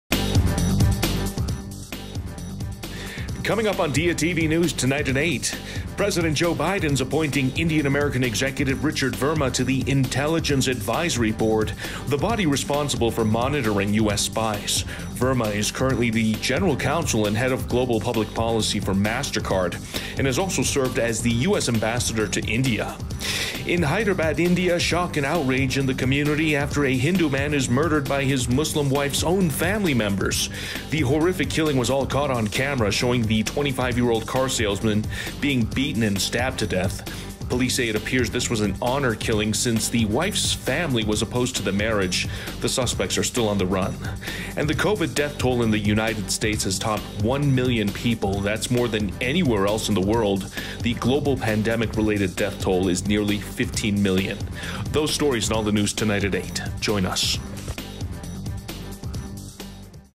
Verma appointed to Intelligence Advisory Board | Diya TV News
Verma appointed to Intelligence Advisory Board

President Joe Biden is appointing Indian-American executive Richard Verma to the Intelligence Advisory Board, the body responsible for monitring US spies.
 Verma is currently the general counsel and head of Global Public Policy for Mastercard and has also served as the US Ambassador to India.

Hindu man married to Muslim woman, killed by her family

In Hyderbad India, shock and outrage in the community after a Hindu man is murdered by his Muslim wife's own family members. The horrific killing was all caught on camera, showing the 25-year-old car salesman being beaten and stabbed to death. Police say it appears this was an honor killing since the wife's family was opposed to the marriage. The suspects are still on the run.

US Covid death toll crosses 1 million

The Covid death toll in the United States has topped 1 million people. That's more than anywhere else in the world. The global pandemic-related death toll is nearly 15 million.

Diya TV ®
Enlighten. Entertain. Inspire.™